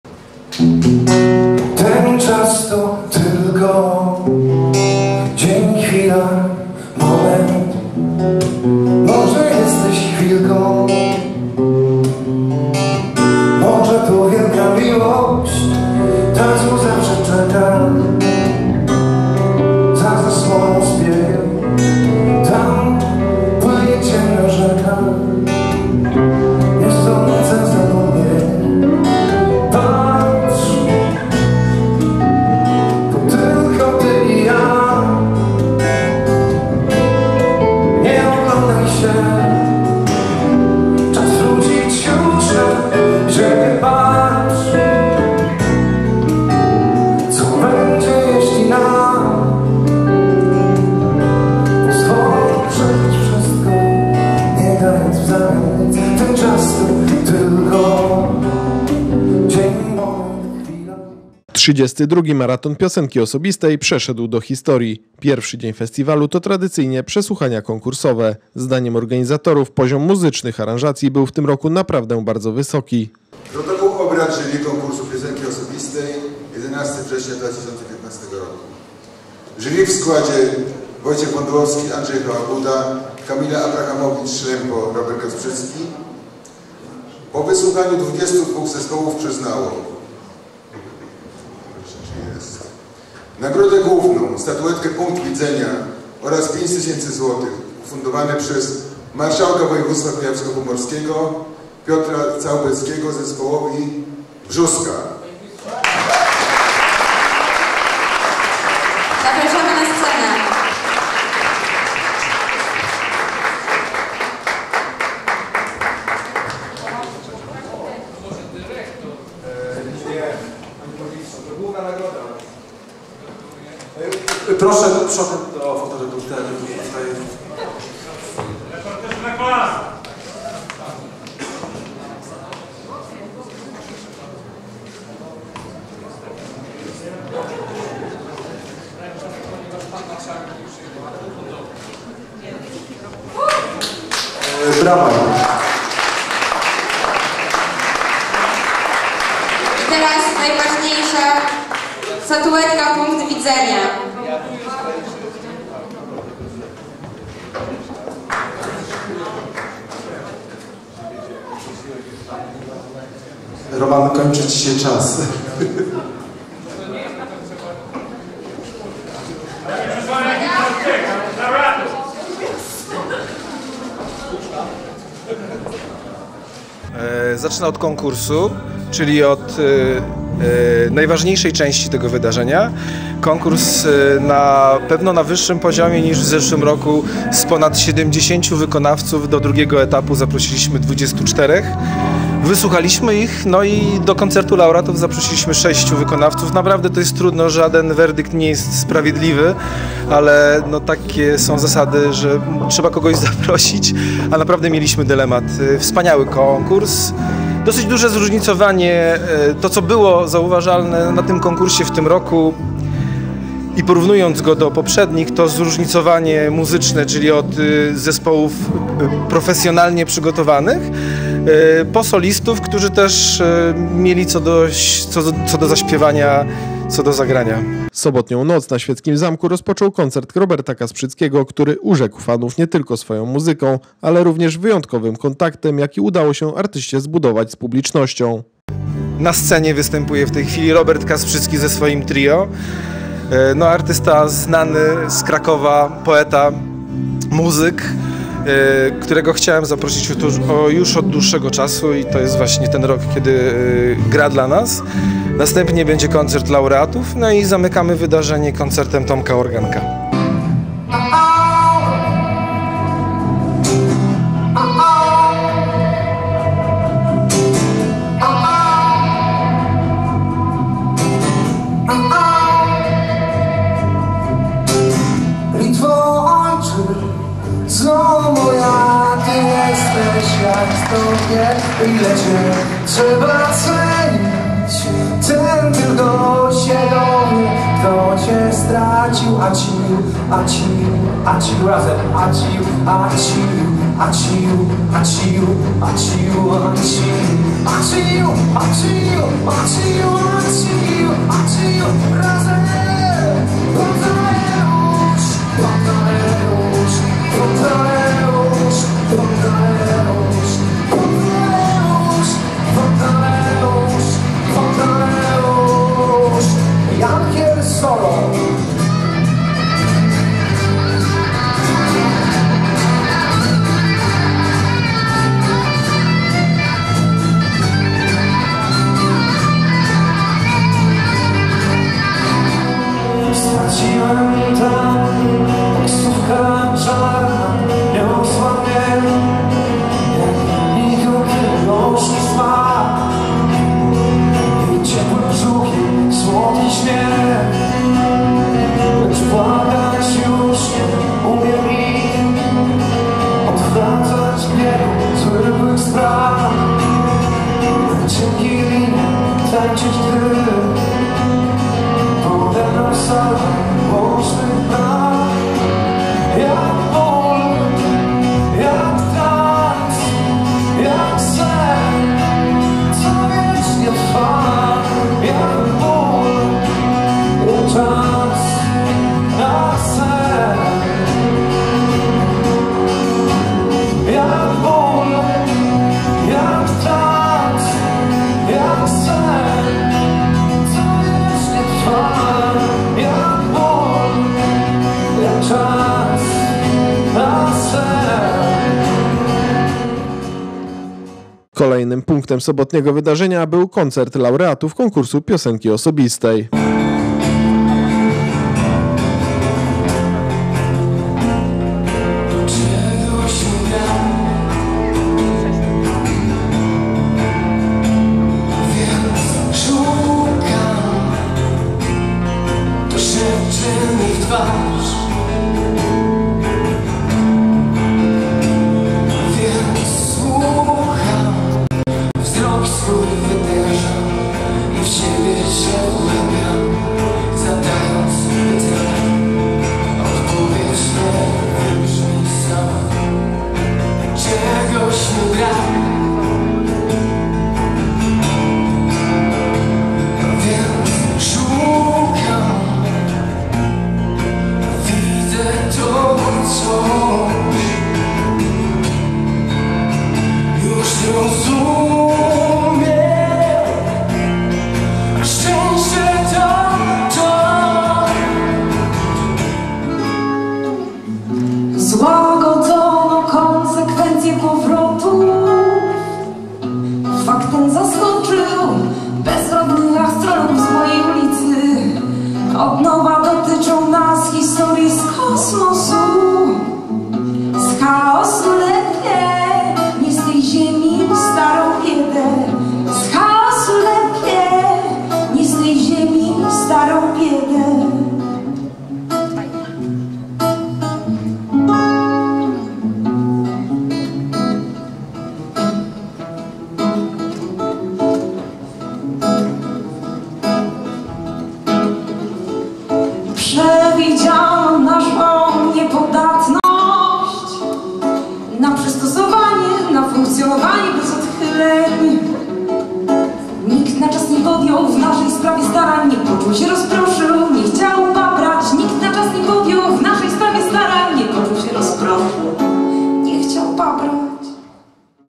TV CSW: Nocne Śpiewanie '2015
Na prośbę naszych widzów udostępniamy w internecie materiał z tegorocznego Maratonu Piosenki Osobistej "Nocne Śpiewanie" .